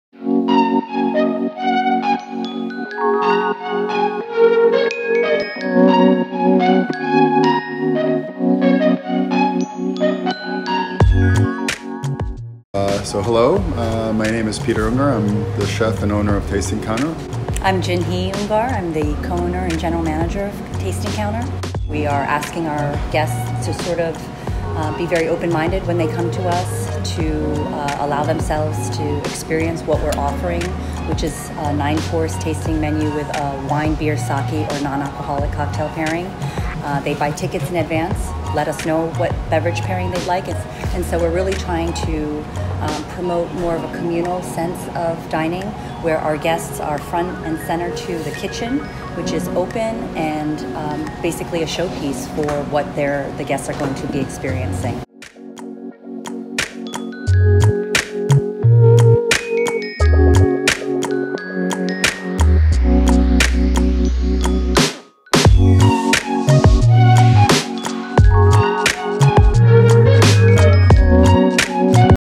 Tasting Counter Taste
Digital Marketing Project at the Boston University School of Hospitality Administration with Leora Lanz, Marut Raval, Jenny Park and Sunhee.
Client: Tasting Counter located in Sommerville, MA.
Chef / Owners Peter Ungár & Ginhee (Ginhee Sohn) Ungar.

Tasting Counter named 2019's BEST RESTAURANT IN BOSTON by Boston Magazine.

"Taste" focusing on the food; "Talk" focusing on the ambiance; "Toast" focusing on the wine pairings, all created in-house.